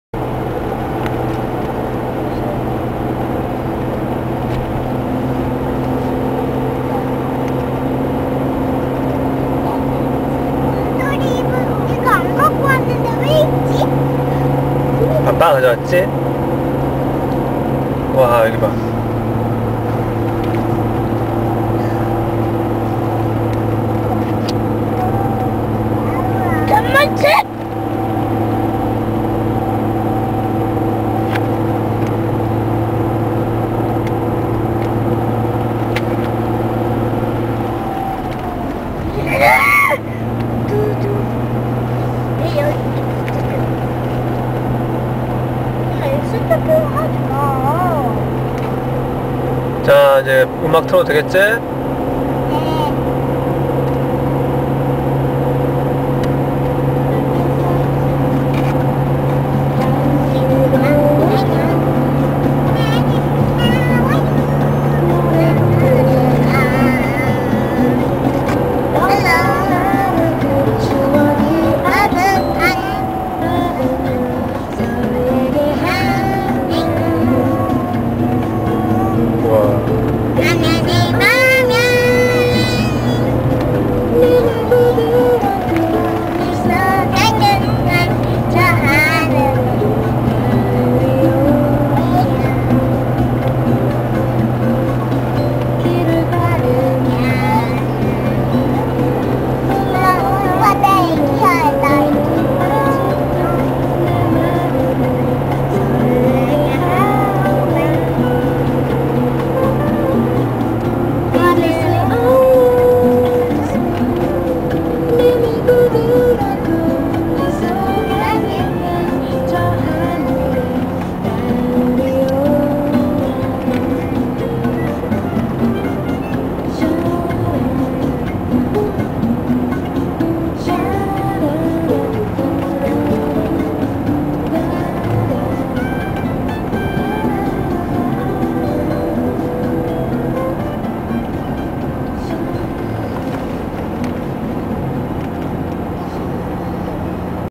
Autumn driving in the X-type 2.5
Autumn Trip with my family in the 2002 X-type 2.5.
Enjoy the scenery of South Korea. ;o)